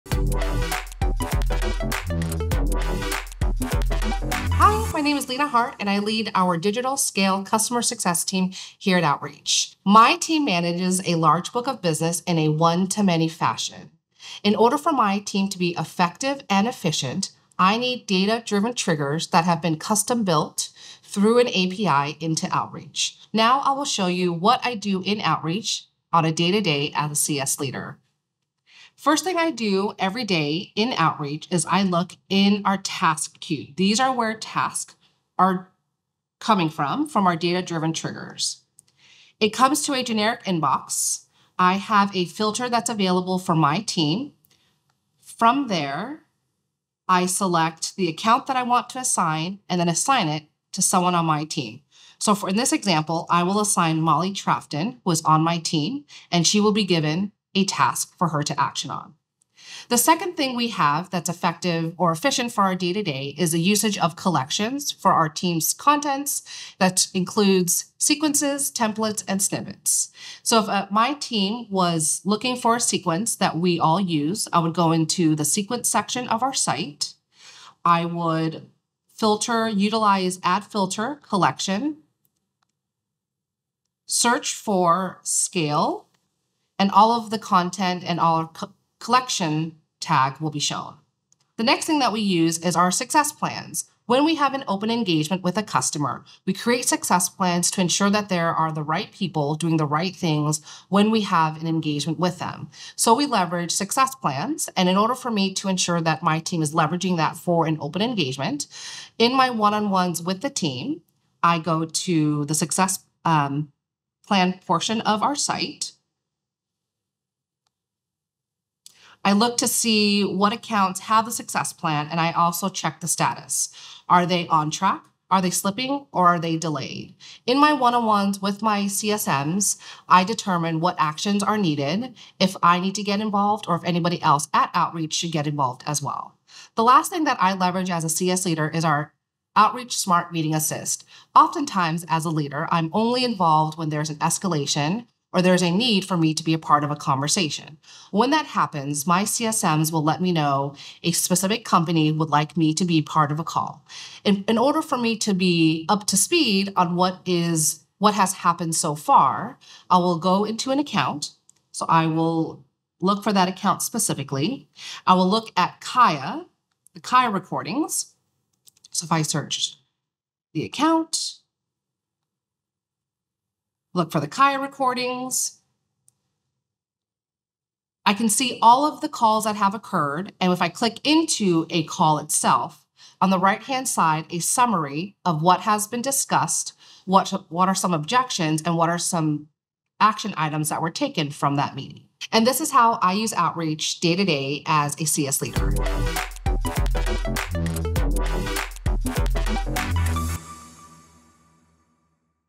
How a Senior CSM uses Outreach
Struggling to manage a large customer success team effectively?

Watch Leana Hart, Digital Customer Success Team Lead at Outreach, reveal her data-driven approach using Outreach to maximize team efficiency and customer engagement.

 In this video, you'll learn:
• How to leverage custom-built API triggers for automated task assignment and workflow.

• How to organize and share team content easily with collections for sequences, templates, and snippets.
• How to track and manage success plans to ensure customer milestones are met.

• How to utilize smart meeting assist to quickly get up to speed on customer interactions.

• How to analyze past conversations with KAIA call recordings and summaries.

By implementing these strategies, you can:
• Scale your customer success team's impact with automation and data-driven insights.

• Improve team collaboration and knowledge sharing.
• Proactively manage customer engagements and drive success.
• Become a customer success leader who thrives in a fast-paced environment.

Watch the video now and unlock the full potential of your digital scale customer success team!